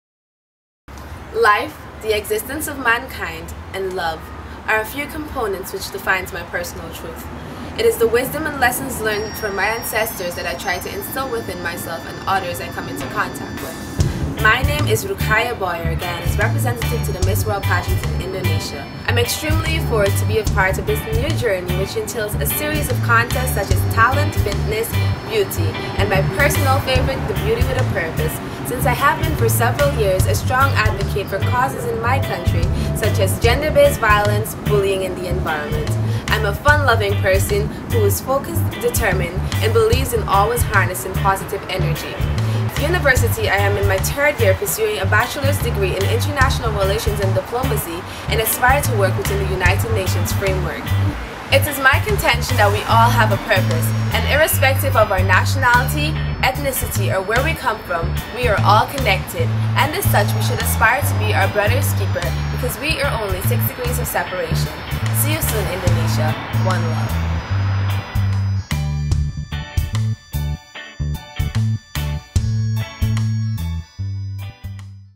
Miss World 2013 - Guyana - Contestant Introduction
An introduction to Ruqayyah Boyer, Guyana's entry to Miss World 2013!